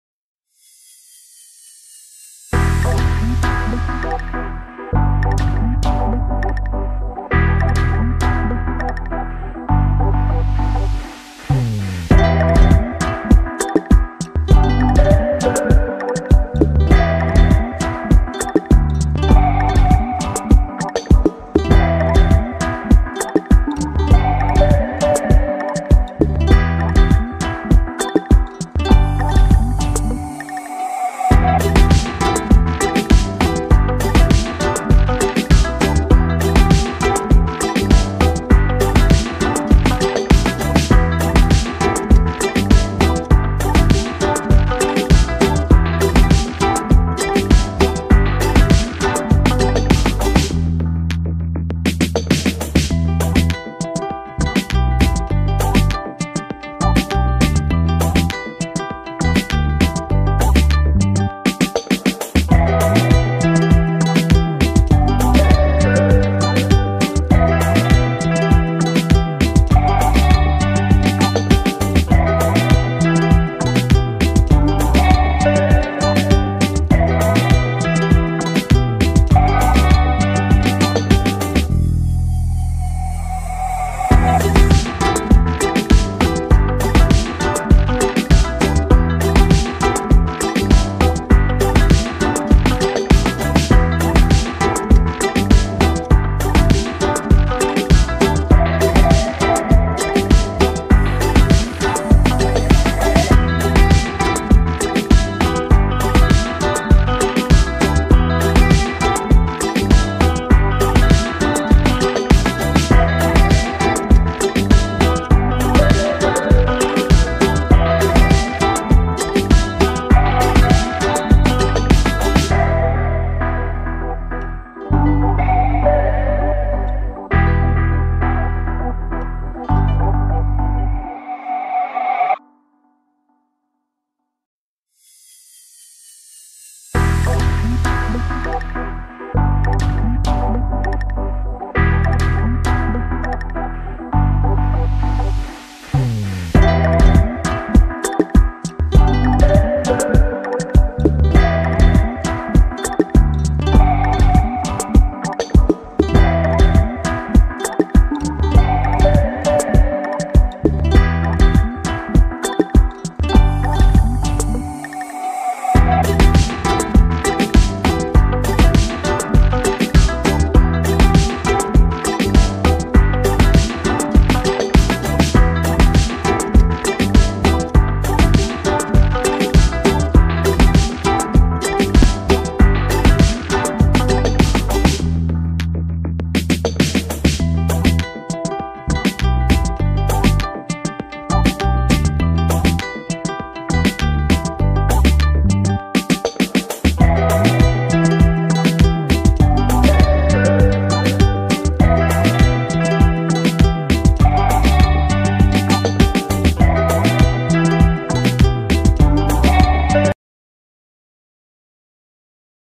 How to Make Paper Drone That can fly || DIY Paper Drone
Hi Guys,In this video we will show you How to Make Paper Drone, It is a very interesting video and you will learn to make DIY Paper Drone That can fly very easily.